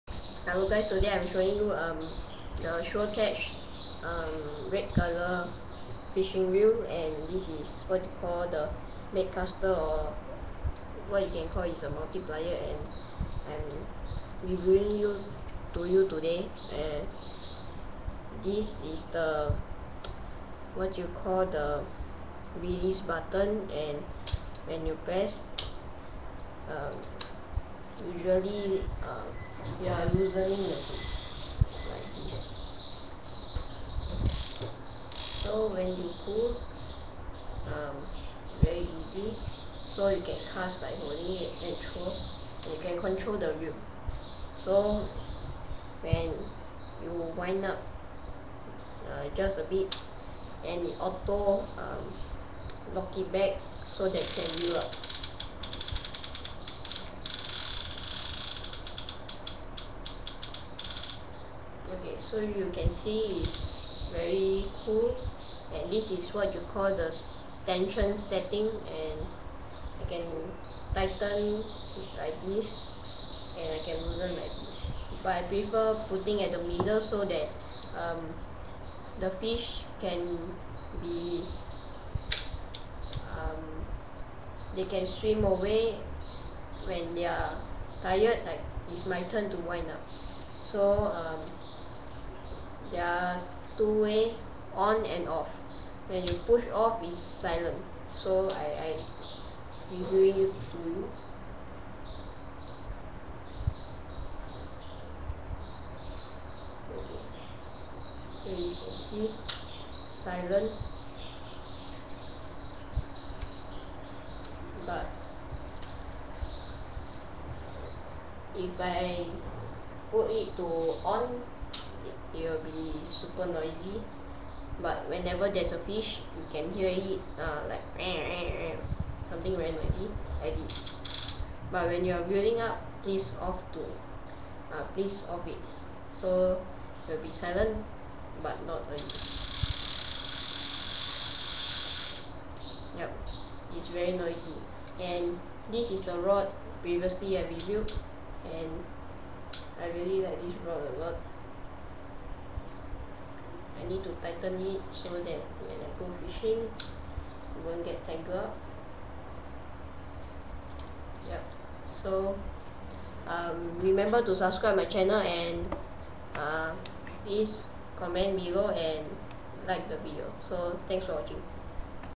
SureCatch bait caster (Red)
basically this fishing reel is curiously awesome and fully metal except the grip and this reel is only found at certain shops only